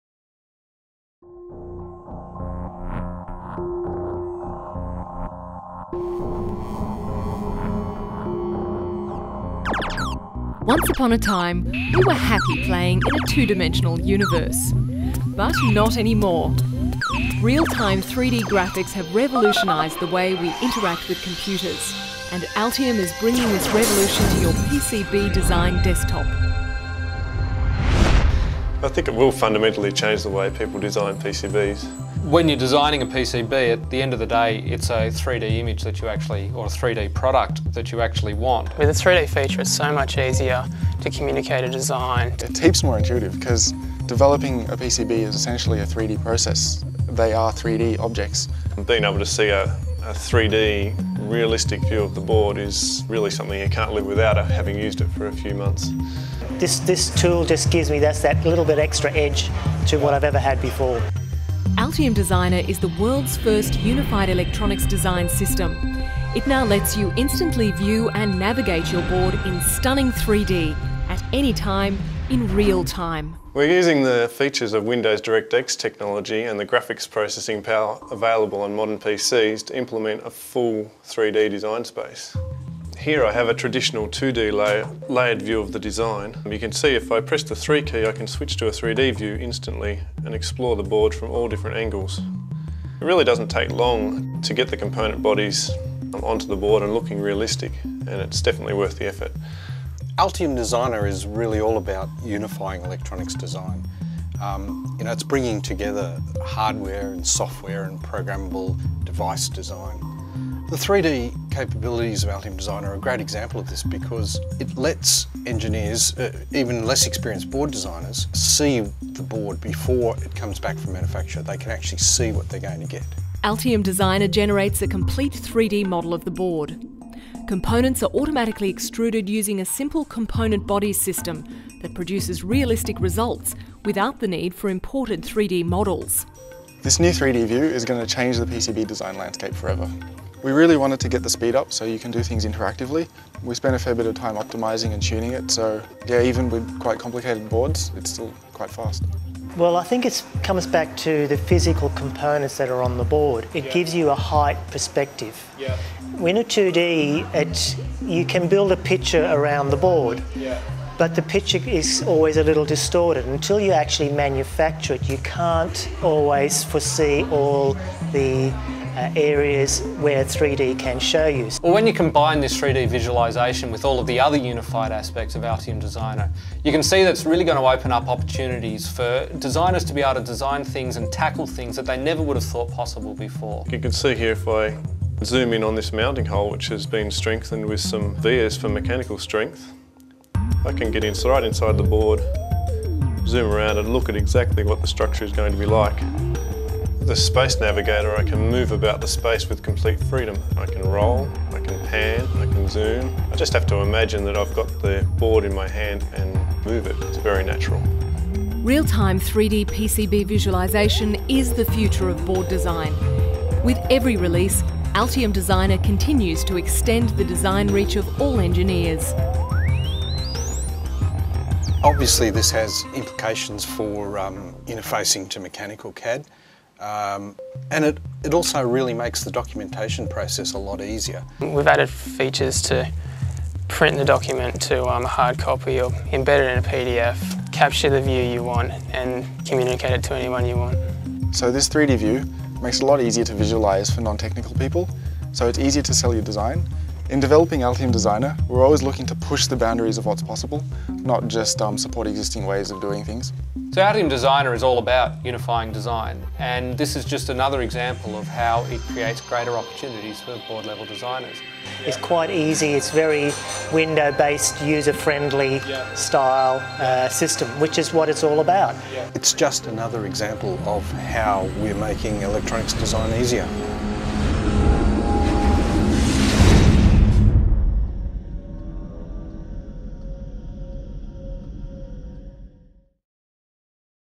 Altium Designer 3D PCB tool Overview. By Dingo Dan Young dingo media
A new concept in PCB electronic design. Altium Designer overview 3d PCB The future of PCB design.3D PCB tool, Altium Designer goes 3D. Created by Dan Young
An in depth, short documentary style look at where electronic design is headed. differentiation, manufacture. Printed Circuit Board.
Created by Dan Young, David Moore and Stanly Huang  for Altium
dingo media